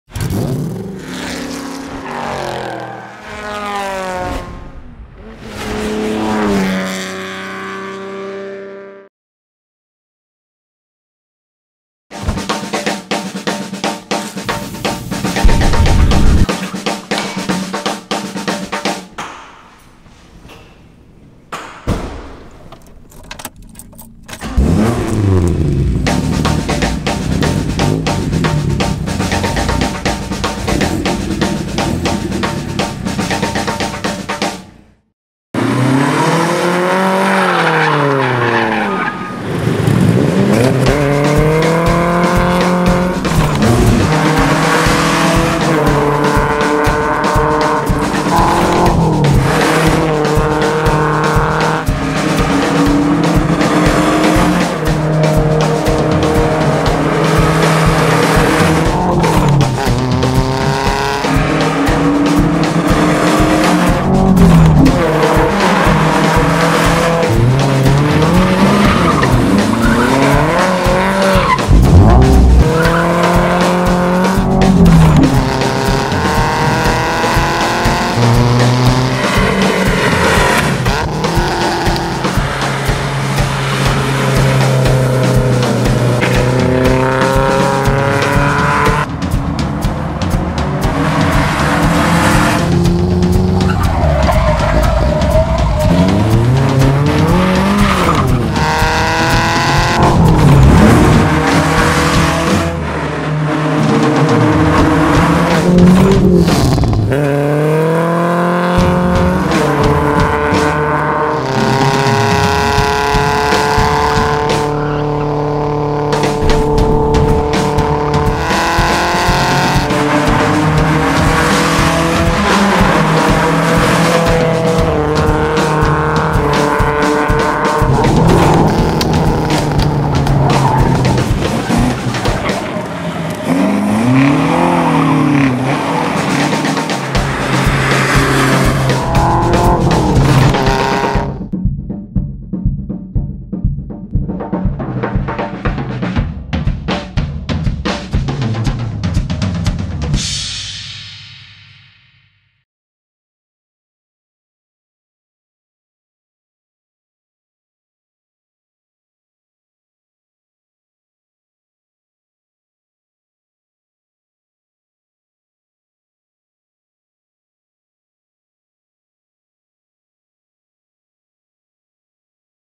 Wake Up With An Espresso Shot Of Alfa Romeo GTV
Cars and coffee make for a common pairing, but we aren’t talking about a rushed cup grabbed at the gas station that’s slugged down on autopilot as you sit in traffic with the commuting masses on Monday morning. The parking lot car shows that have taken on the Cars & Coffee name are a better way to enjoy the two, but this is a different story altogether. This is one of those perfect mornings when a quality roast is followed by an equally caffeinated drive away from the gridlock of silver and beige, in a car that could easily pass for the automotive manifestation of espresso. Find an empty street, add a hot-rodded Alfa Romeo GTV, and try not to have the time of your life.

Drive Tastefully®